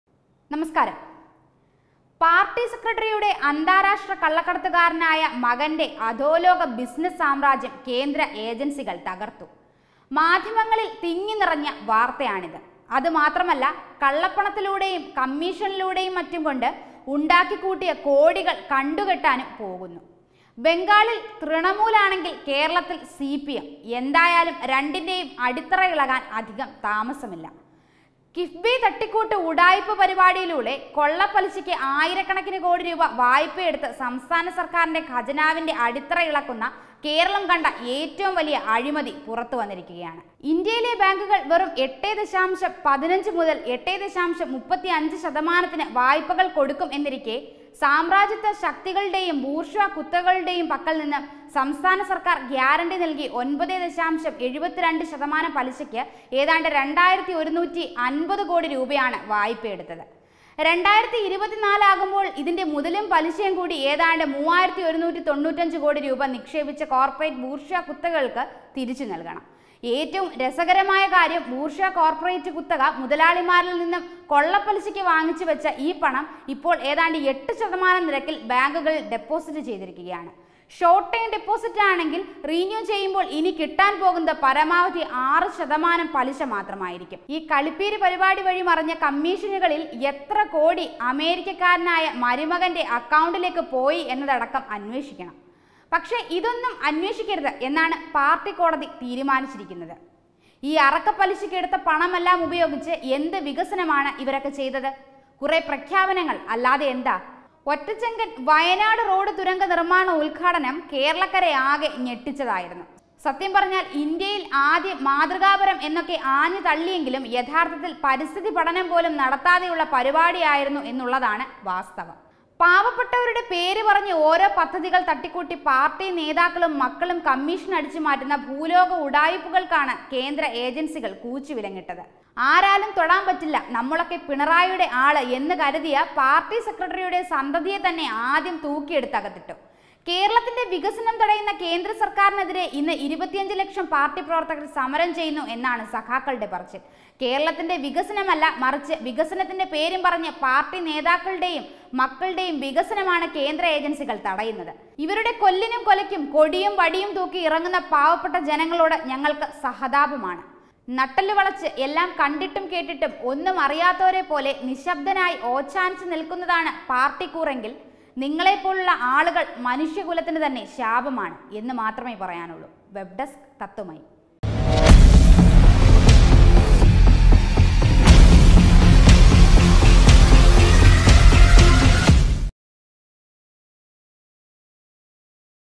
ഓച്ഛാനിച്ച് നിൽക്കുന്നതാണോ പാർട്ടി കൂറ്? സർക്കാരിന്റെ ഉഡായിപ്പുകൾക്ക് കൂച്ചുവിലങ്ങിട്ട് കേന്ദ്രം
ഓച്ഛാനിച്ച് നിൽക്കുന്നതാണോ പാർട്ടി കൂറ്? സർക്കാരിന്റെ ഉഡായിപ്പുകൾക്ക് കൂച്ചുവിലങ്ങിട്ട് കേന്ദ്രം | CPM